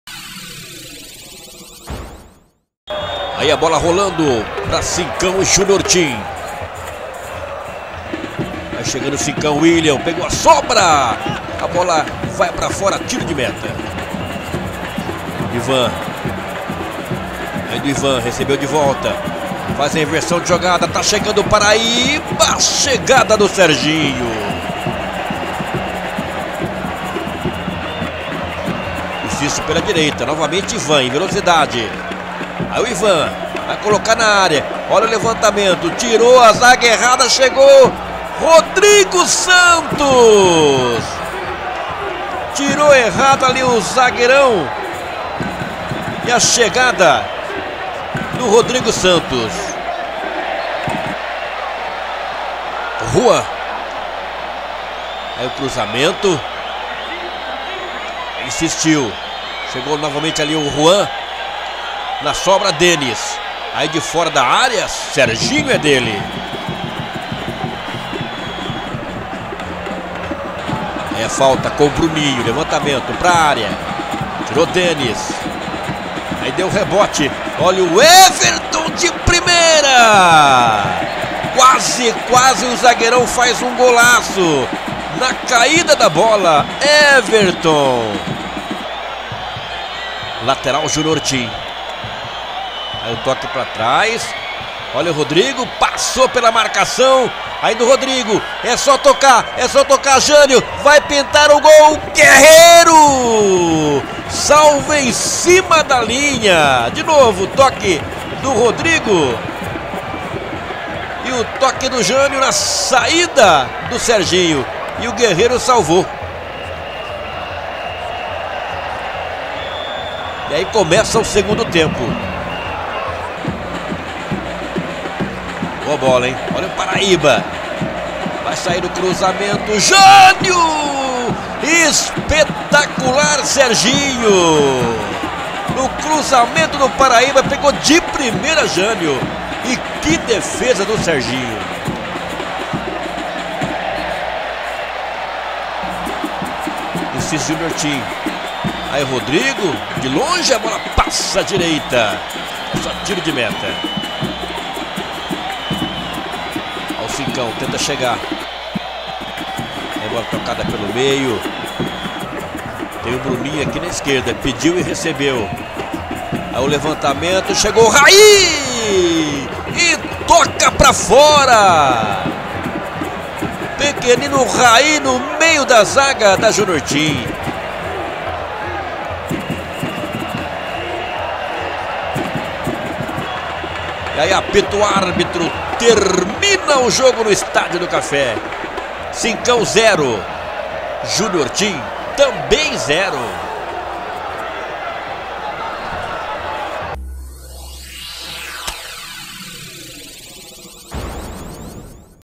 CINCÃO E.C. 0X0 JR. TEAM-SERTANÓPOLIS
HEXAGONAL FINAL DA DIVISÃO DE ACESSO PARANAENSE DE 2013. JOGO REALIZADO NO ESTÁDIO DO CAFÉ DIA 17.07.13. CONFIRA OS MELHORES MOMENTOS DO JOGO COM IMAGENS DA SPORTVÍDEO E NARRAÇÃO DE ALTAIR ANDRADE.